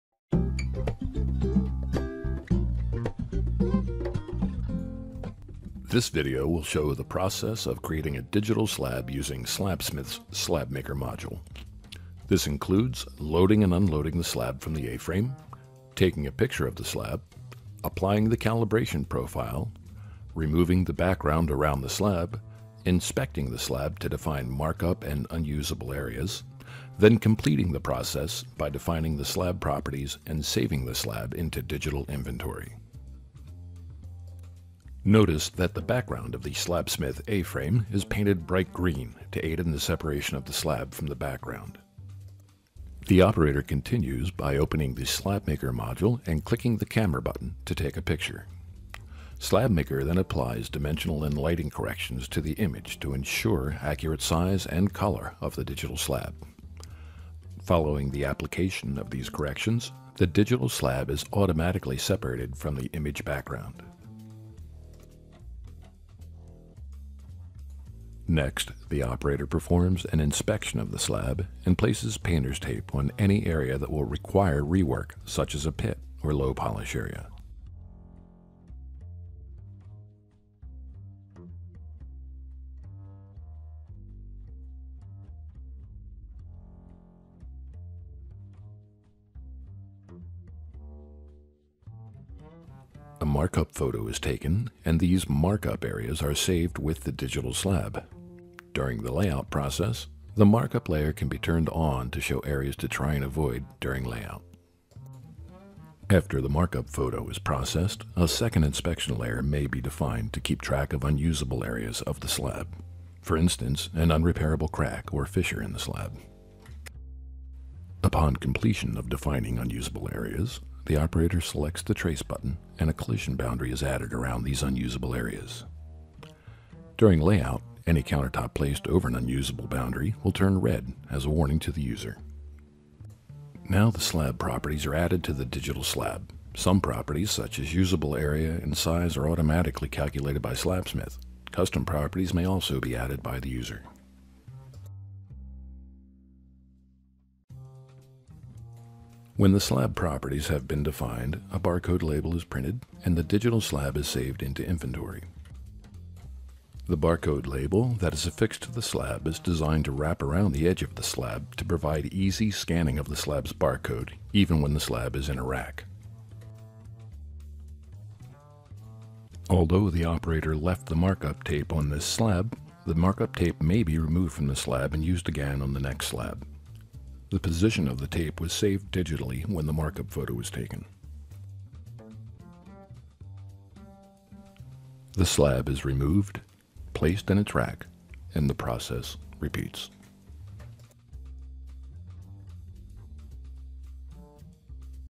Slabsmith™ - Photographing Slabs with 'Slab Maker™'
A video showing a Slabsmith customer loading a slab on the Slabsmith photostation A-frame, creating a digital slab, creating inspection layers,  assigning slab properties and saving the slab into digital inventory.